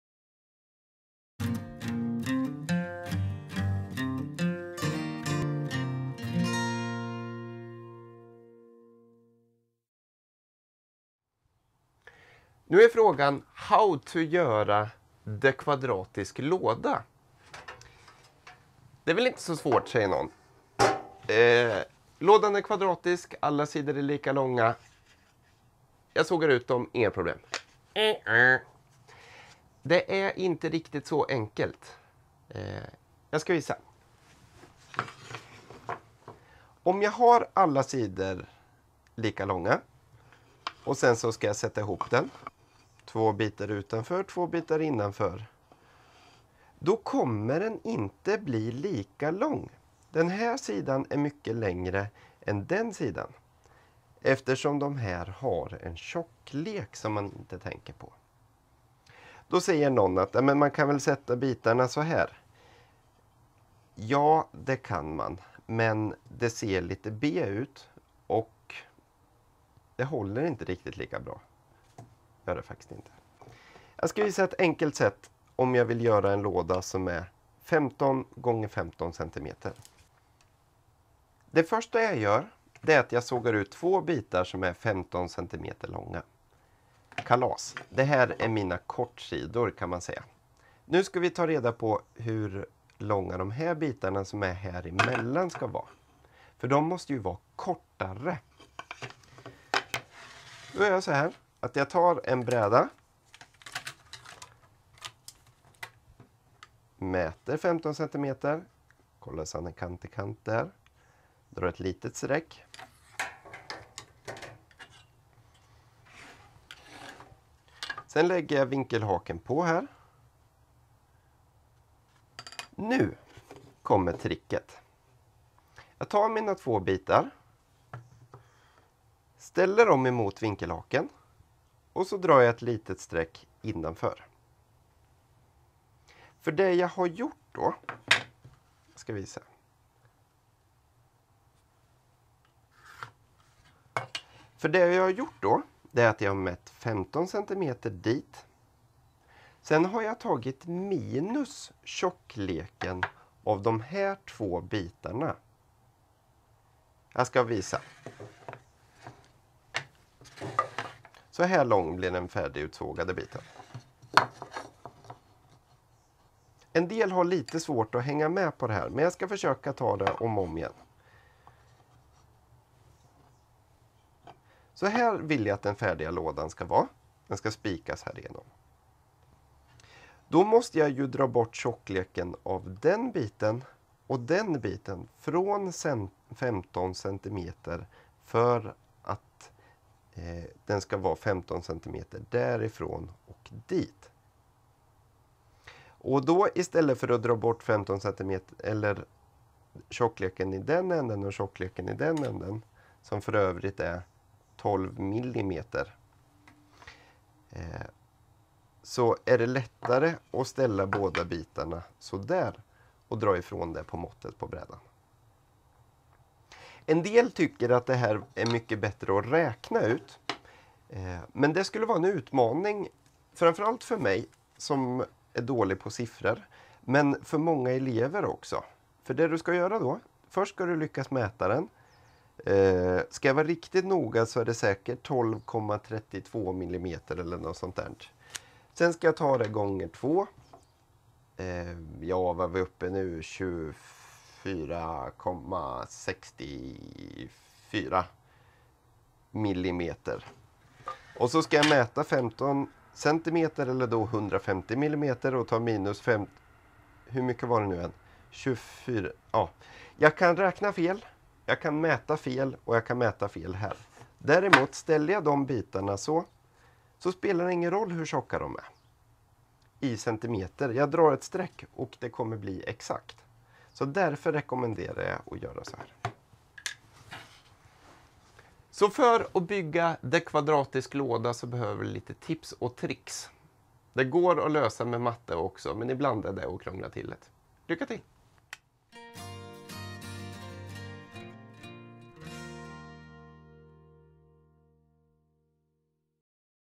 Kvadratisk låda Utvändiga mått (Bygg en låda)
Hur du ska mäta bitarna till en kvadratisk låda med ett särskilt utvändigt mått.